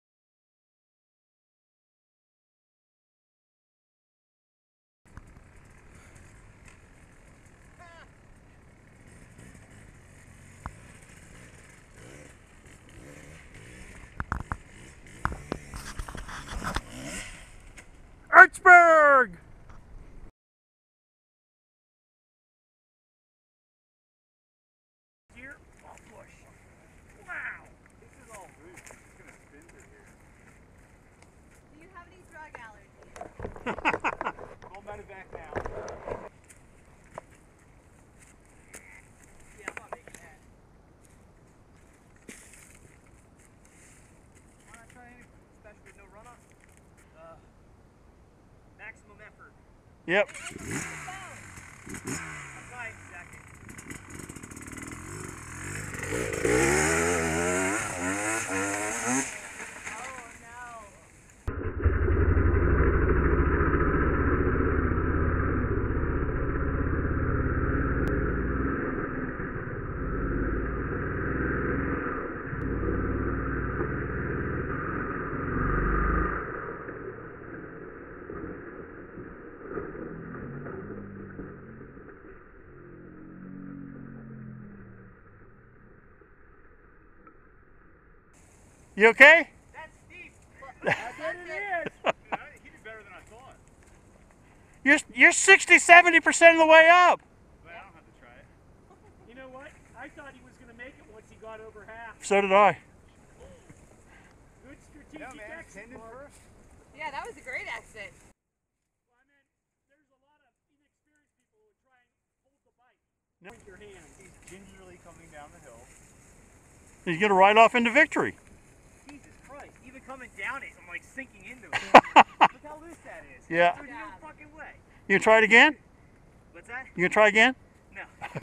FRO LocusGap, Mark tries some hill climbs
Two quick clips of Mark Stainthorpe attempting climbs on the eastern end of the Locus Gap trail system. Channeling his inner Graham Jarvis